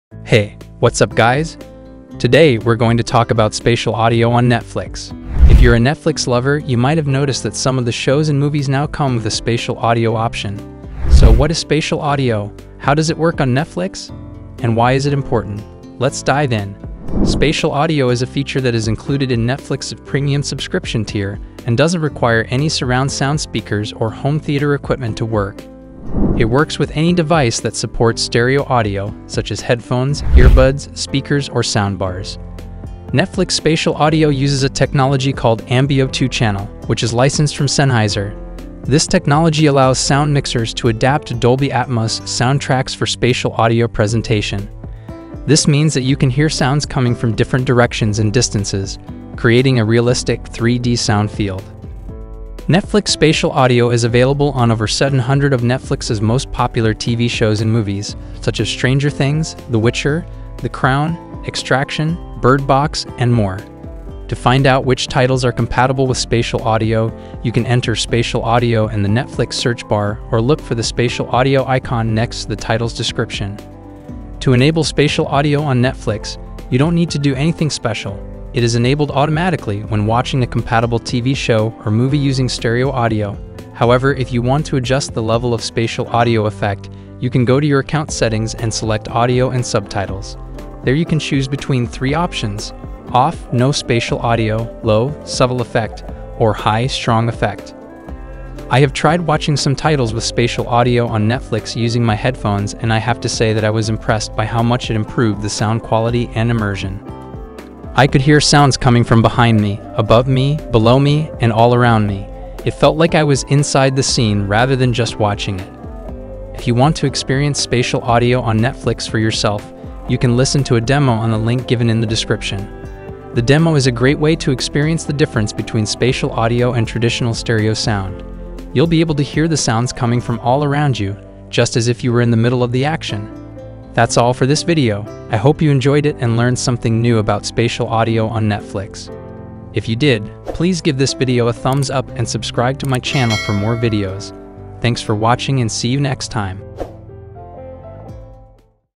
Spatial Audio on Netflix What Is It and How Does It Work
Want to dive deeper into the world of Spatial Audio on Netflix? We've got you covered! In addition to our video, we've written an in-depth guide on this topic, exploring how it works and how you can make the most of it. Enhance your Netflix experience by checking out our comprehensive guide
Looking for a way to enhance your Netflix viewing experience? Check out this video on Netflix Spatial Audio! Learn everything you need to know about this cutting-edge audio technology, including how to enable it on your device, and discover some of the best Netflix movies to enjoy with Spatial Audio. From AirPods Pro to iPhone, we cover it all. Don't miss out on this Spatial Audio test and take your Netflix binge-watching to the next level!

In this video, we'll take a deep dive into Netflix's Spatial Audio feature and explore how it works. We'll show you step-by-step how to enable Spatial Audio on your device, whether you're using an iPhone, iPad, or other compatible device.

We'll also explore some of the best Netflix movies that are optimized for Spatial Audio, so you can immerse yourself in a truly cinematic experience. From action-packed blockbusters to heartwarming dramas, there's something for everyone on this list.

And if you're wondering whether your AirPods Pro or other headphones are compatible with Spatial Audio, we've got you covered. We'll walk you through everything you need to know to enjoy this feature to the fullest.

Whether you're a Netflix fanatic or just looking for a way to enhance your movie-watching experience, this video is for you. So sit back, relax, and prepare to be blown away by the power of Spatial Audio!
WHAT'S THE DIFFERENCE? New Netflix Spatial Audio vs Atmos